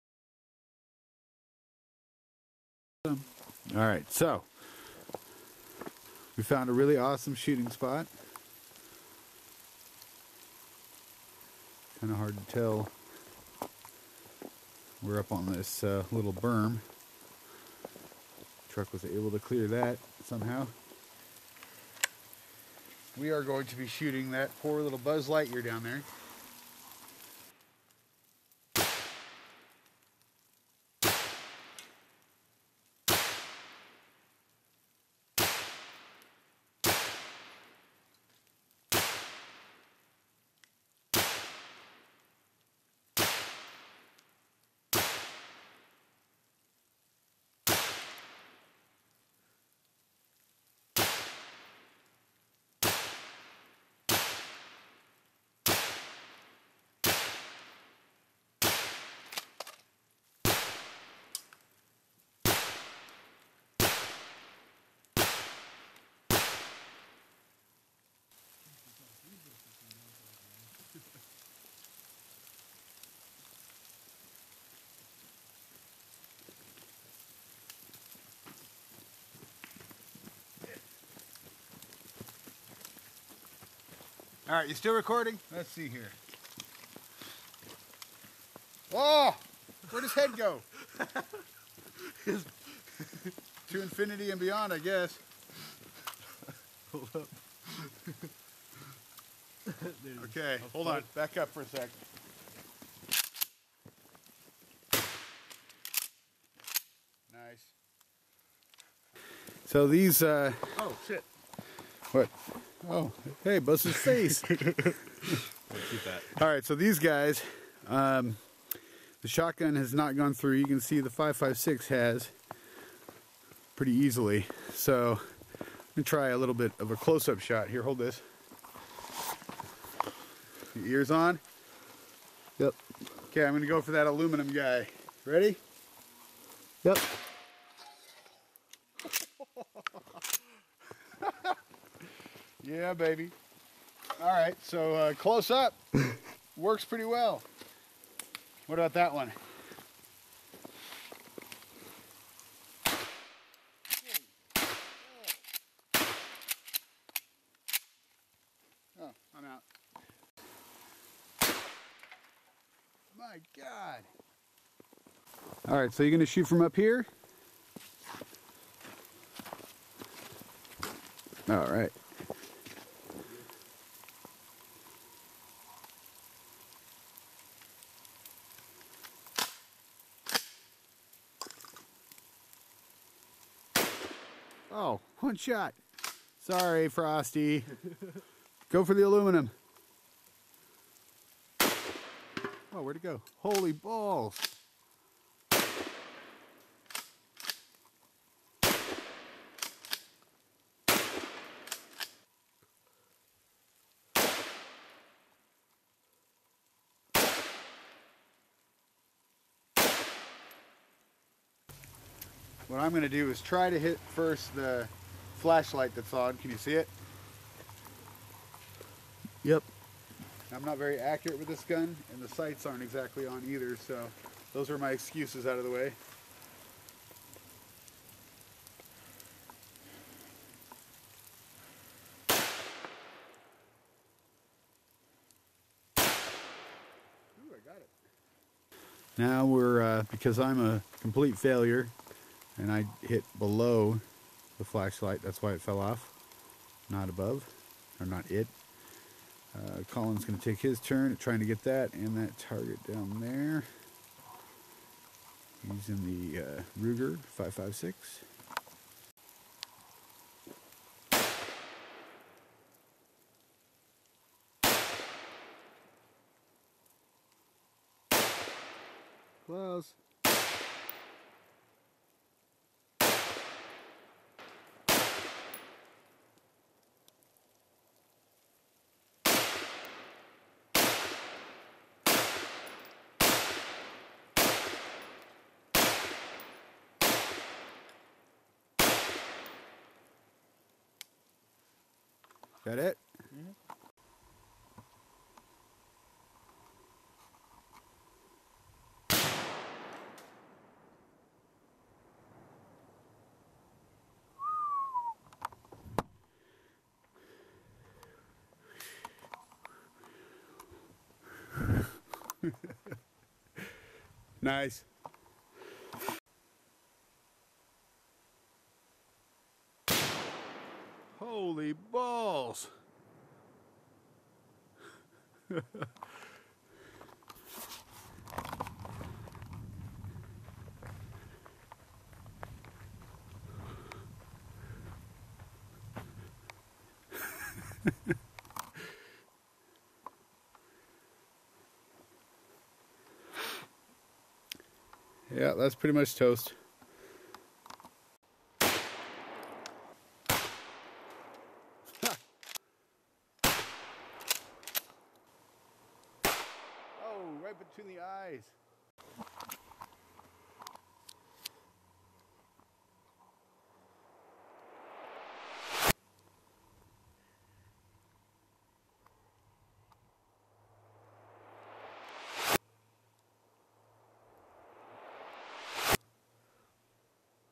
Enjoying nature
Colin Hawkins and I enjoying some nature time.  Playing with a couple of 12 gauges, and my Ruger556.  Plus a couple of clips fro this last summer.